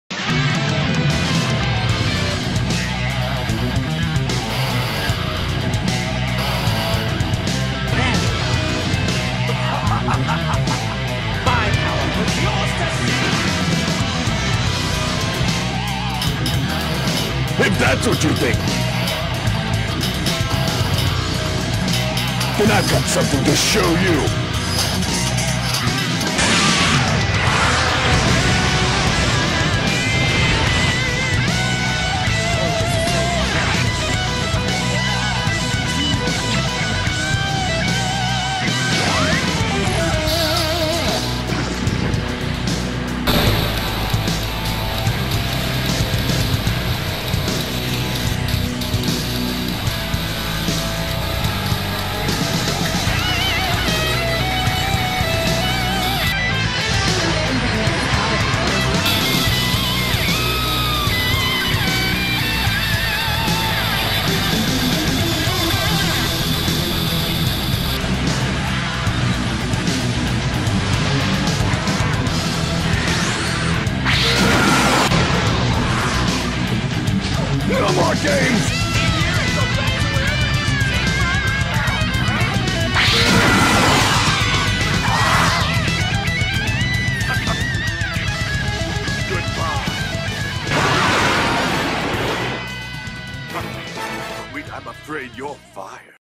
Saiyan Saga Vegeta VS Cui(ベジータVSクイ)
Idea By:Me

Cui has sent to Earth as well to punish Vegeta for betraying Frieza! Can the Saiyan prevail1